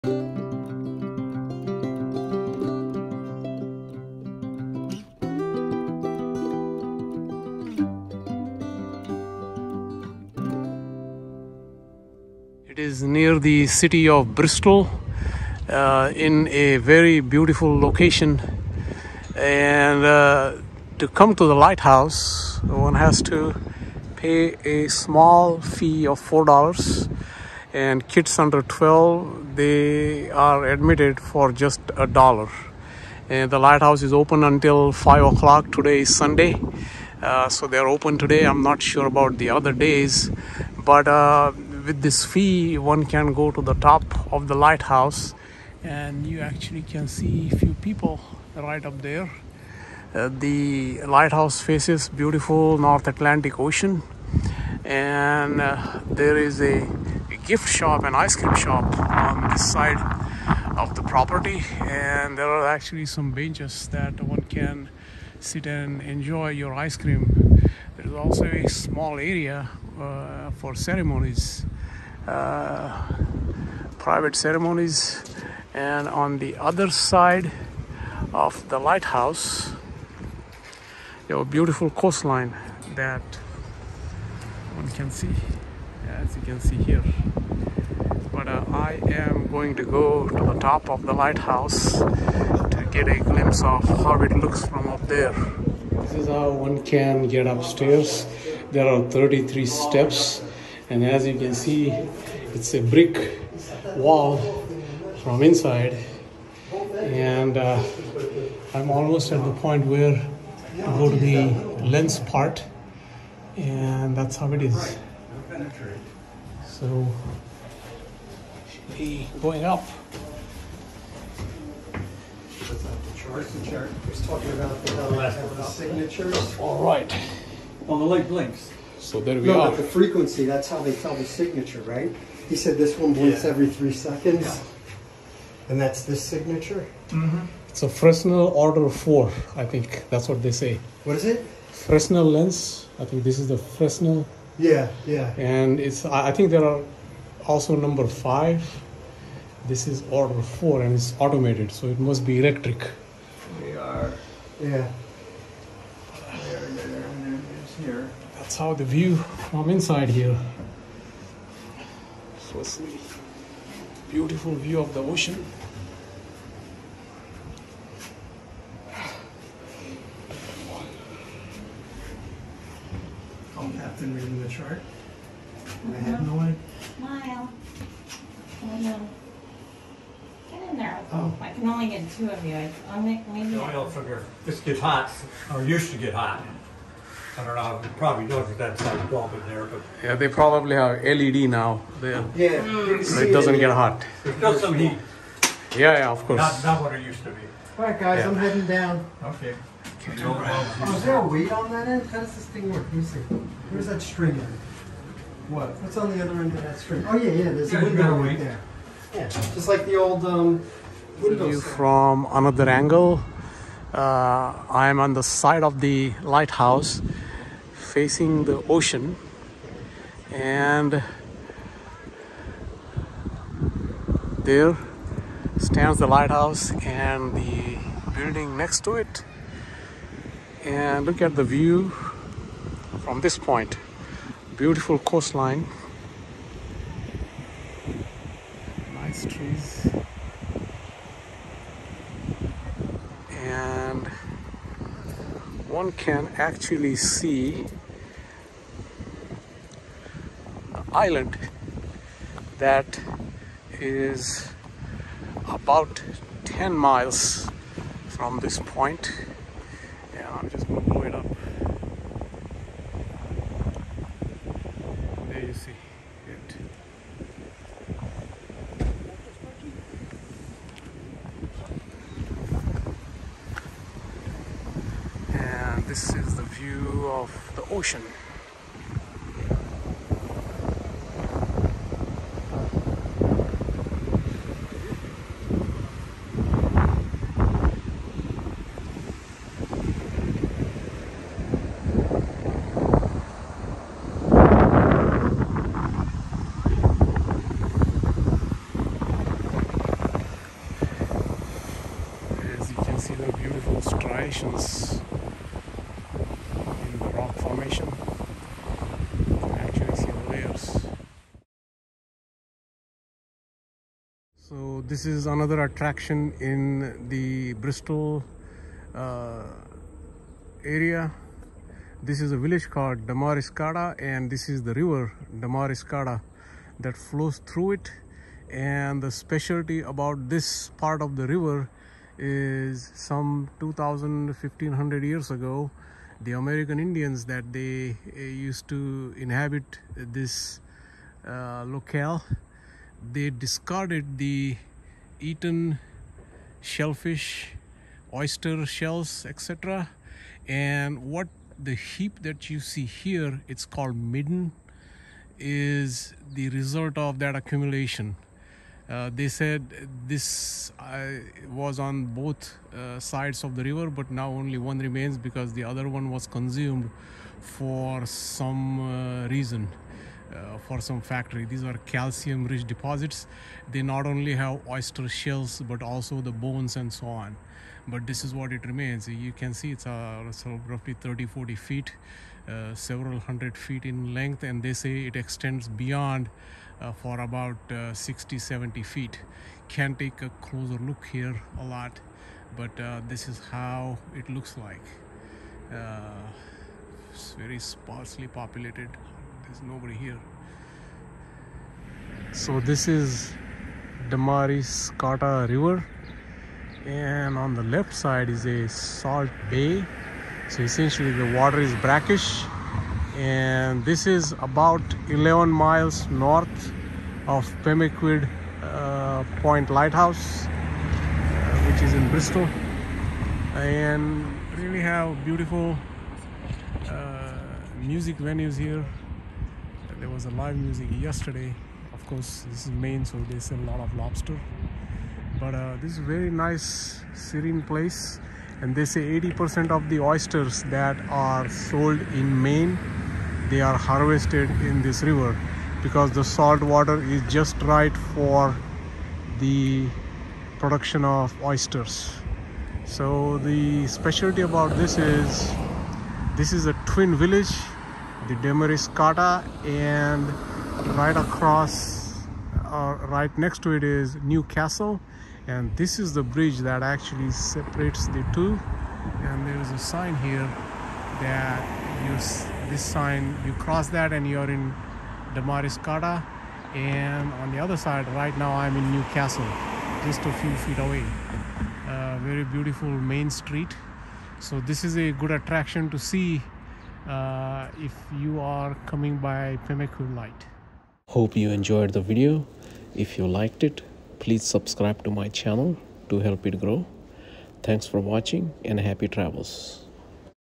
Lovely mid-coast Maine Pemaquid Point lighthouse and nearby attractions
Perched on rugged cliffs, Pemaquid Lighthouse stands as a historic sentinel guarding Maine's scenic mid-coast.  It is about 175 miles north of Boston and 60 miles east of Portland, Maine.

Pemaquid Lighthouse holds a rich history dating back to 1827 when it was commissioned by John Quincy Adams.
The original Lighthouse was a 38-foot rubblestone tower with a fixed light. In 1857, it was replaced by a taller, more prominent tower made of brick. This new tower featured a fourth-order Fresnel lens.
In 1934, the light was electrified, replacing the traditional kerosene lamp, and shines 14 miles on a clear day!

Damariscotta Town is nestled along the Damariscotta River, about 10 miles north of Pemaquid Lighhouse. This charming Maine town had 2200 residents at last census and exudes New England quaintness. With its vibrant arts scene, historic downtown, friendly community and river backdrop, Damariscotta is a hidden gem waiting to be explored.